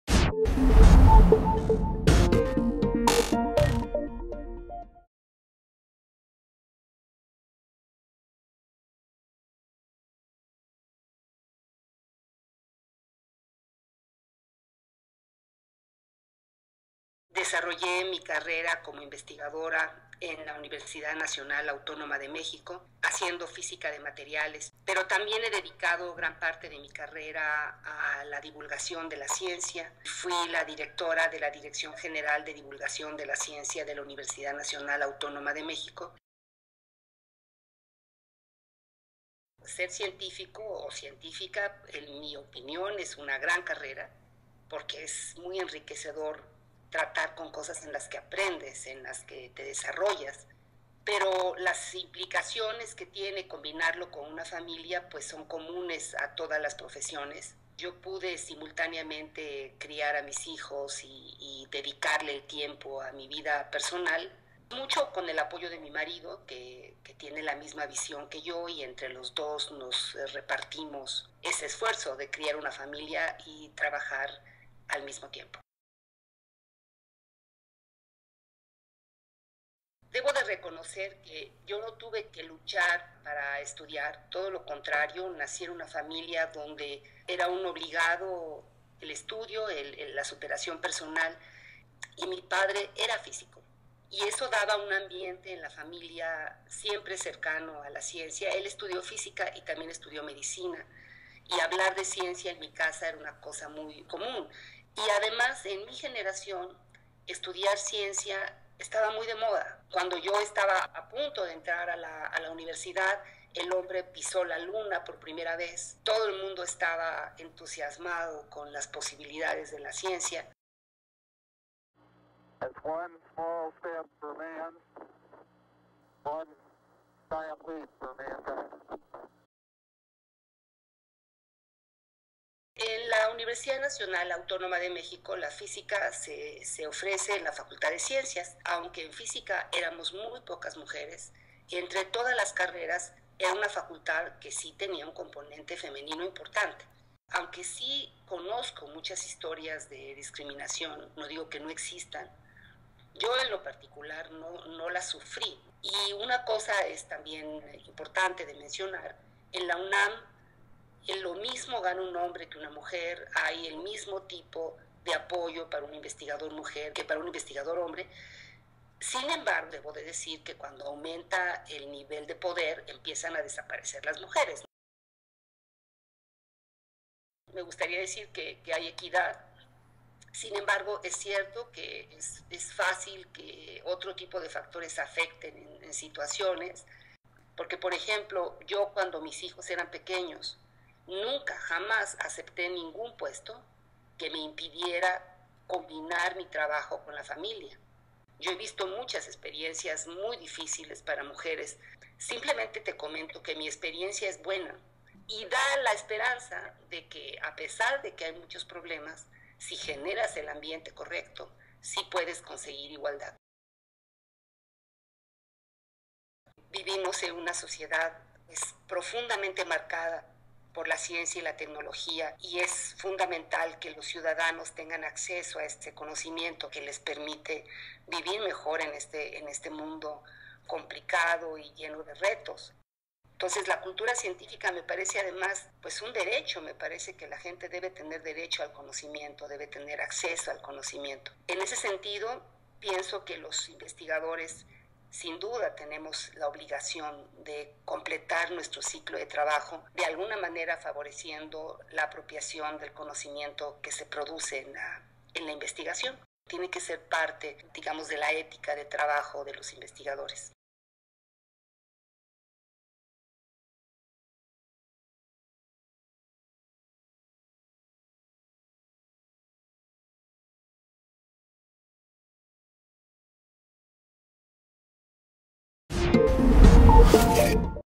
Julia Tagüeña: Gender equality and promoting science
With more than 25 years' experience, Julia Tagüeña is one of the most renowned Mexican scientists of her generation. She began her career at the National Autonomous University of Mexico (UNAM), where she did research in materials physics. Since then, she has also worked to promote science.

Tagüeña led UNAM's General Directorate of Science Popularization. She was also director of Red-POP, the Latin American and Caribbean network for science and technology popularisation,

Tagüeña is currently the deputy director for scientific development at Mexico's National Council of Science and Technology.

In this interview, she talks about women's role in Mexican science, the challenges Mexico faces in promoting gender equality and why researchers should do more to spread knowledge to non-scientists.